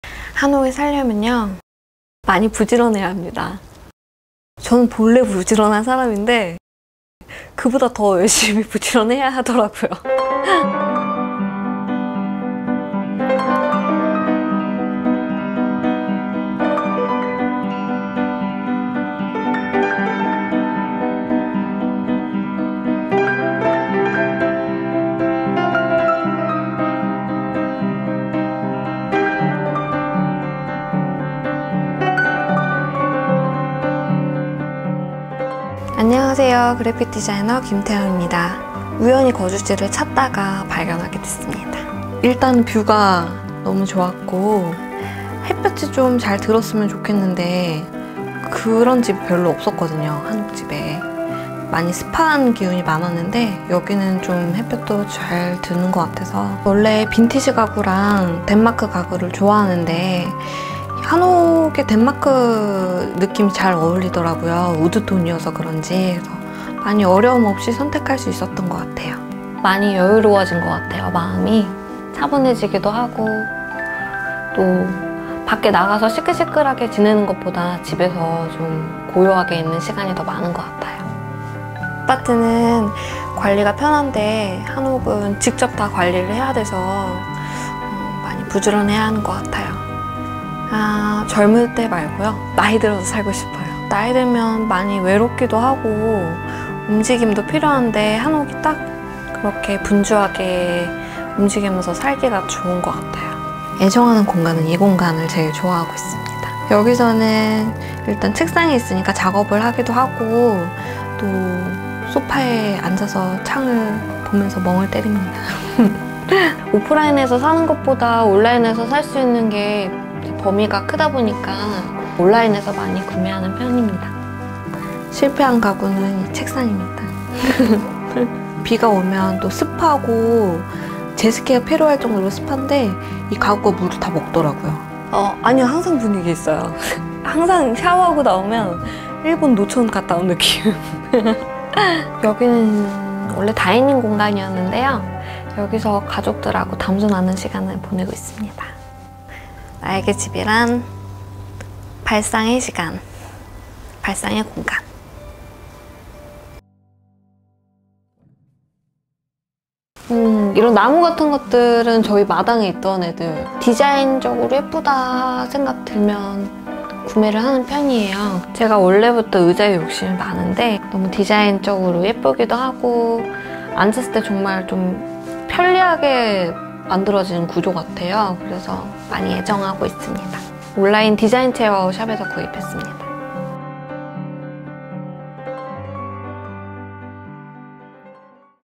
한옥에 살면 힘든 사람이 있어요!
세 번째 홈터뷰 주인공은 그래픽 디자이너, 김태영의 한옥'입니다.
햇살, 바람, 산새, 구름이 놀다가는 한옥 .
여러분들도 잠시 놀다가세요. 소리는 꼭 크게 트시구요 :)

digital director 박애나
film 이명수
makeup 정지은